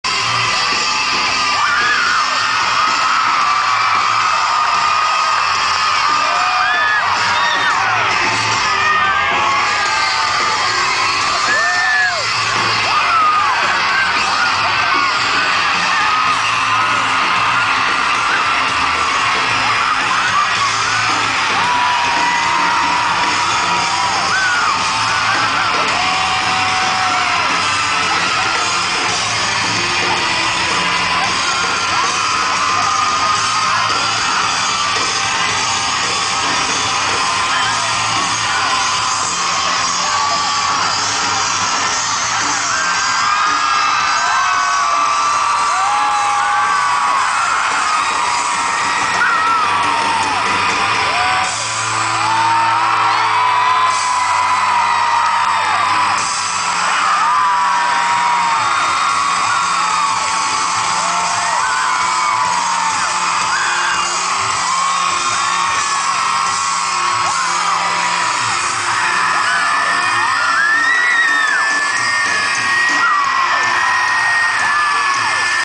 Andrew McMahon of Jack's Mannequin Crowd Surfing
he surfed to the back of the electric factory, rubbed his junk in the security guards faces they crowd surfed back...AMAZING